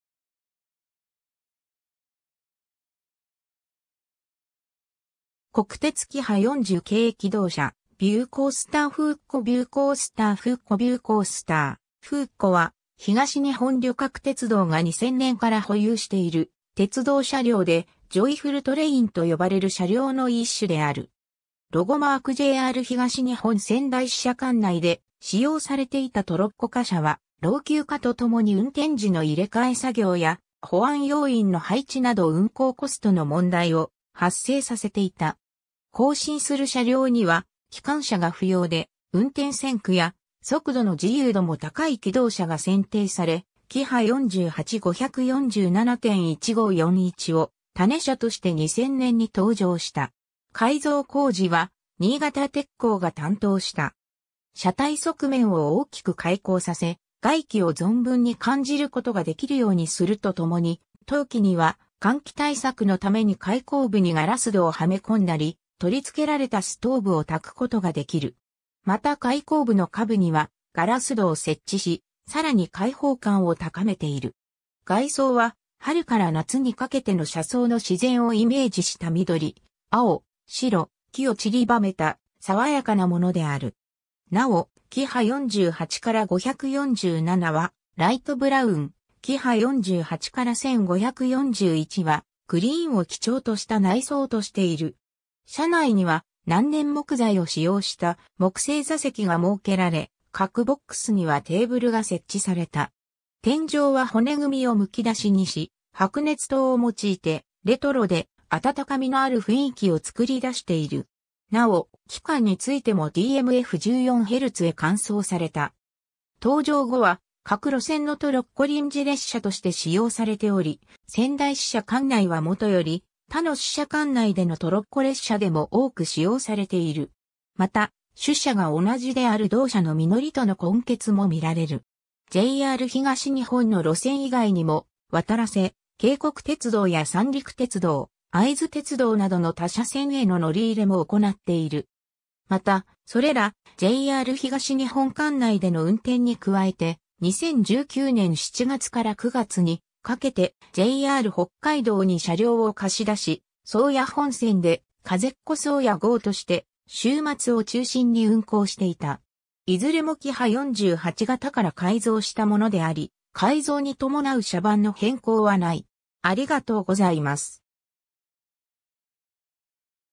びゅうコースター風っこ
国鉄キハ40系気動車 (2代) 」 びゅうコースター風っこ びゅうコースター風っこ びゅうコースター風っこ（ - かぜっこ）は、東日本旅客鉄道（JR東日本）が2000年（平成12年）から保有している鉄道車両で、ジョイフルトレインと呼ばれる車両の一種である。
 ロゴマーク JR東日本仙台支社管内で使用されていたトロッコ貨車は、老朽化とともに運転時の入れ換え作業や保安要員の配置など運行コストの問題を発生させていた。
更新する車両には、機関車が不要で運転線区や速度の自由度も高い気動車が選定され、キハ48 547・1541を種車として2000年（平成12年）に登場した。
改造工事は新潟鐵工が担当した。
 車体側面を大きく開口させ、外気を存分に感じることができるようにするとともに、冬季には寒気対策のために開口部にガラス戸をはめ込んだり、取り付けられたストーブを焚くことができる。
また開口部の下部にはガラス戸を設置し、さらに開放感を高めている。
外装は、春から夏に掛けての車窓の自然をイメージした緑（若葉）、青（川・湖）、白（雲）、黄（光）をちりばめた爽やかなものである。
なお、キハ48-547はライトブラウン、キハ48-1541はグリーンを基調とした内装としている。
車内には、難燃木材を使用した木製座席が設けられ、各ボックスにはテーブルが設置された。
天井は骨組みを剥き出しにし、白熱灯を用いてレトロで暖かみのある雰囲気を創り出している。
 なお、機関についてもDMF14HZへ換装された。
 登場後は各路線のトロッコ臨時列車として使用されており、仙台支社管内はもとより、他の支社管内でのトロッコ列車でも多く使用されている。
また、種車が同じである同社のみのりとの混結も見られる。
 JR東日本の路線以外にも、わたらせ渓谷鐵道や三陸鉄道、会津鉄道などの他社線への乗り入れも行っている。
 また、それらJR東日本管内での運転に加えて、2019年7月 - 9月にかけてJR北海道に車両を貸し出し、宗谷本線で「風っこ そうや」号として週末を中心に運行していた。
 いずれもキハ48形から改造したものであり、改造に伴う車番の変更はない。